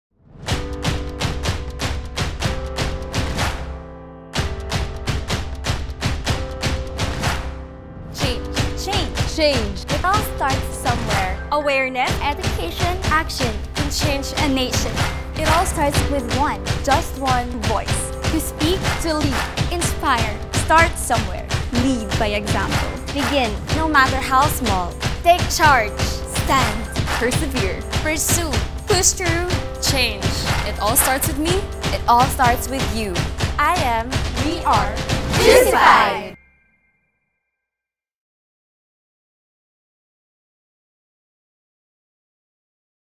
Juicy-fied Teen Squad 2018
Change starts somewhere and our Top 16 Juicy-fied Teens are ready to take on the challenge of spreading sweetness and initiating change through their advocacies!

OFFICIAL CLOTHING PARTNER: Salty Lemon PH